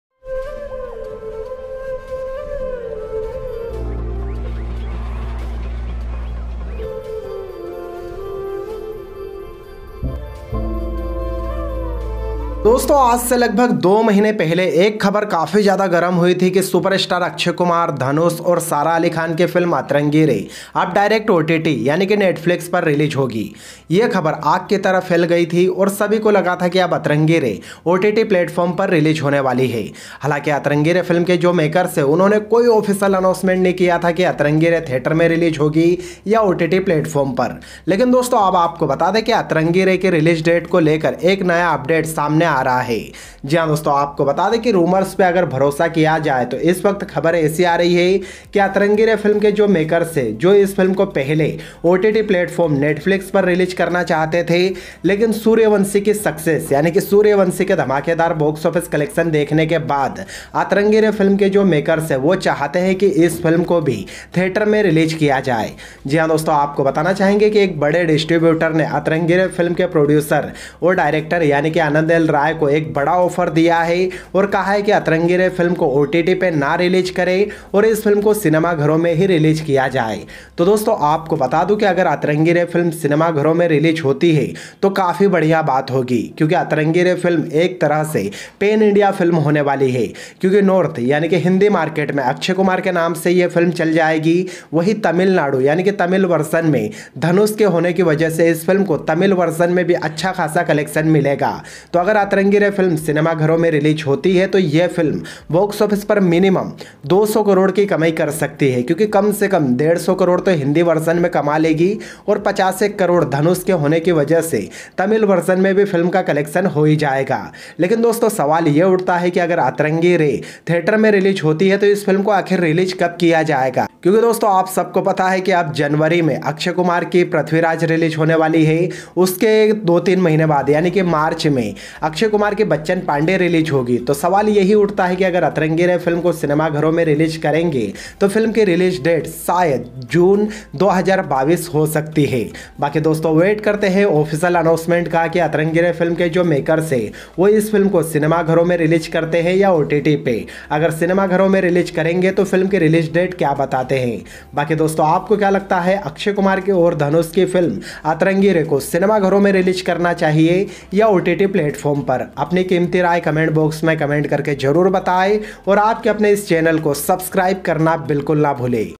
Atrangi Re Movie | Akshay Kumar, Dhanush, Sara Ali Khan, Release Date Update
Atrangi Re Movie Release Exclusive Update

Atrangi Movie - Akshay Kumar, Dhanush, Sara Ali Khan,
Directed By- Anand L Ray,
Producer- T-series

Follow Me In Instagram & Twitter- @iAklesh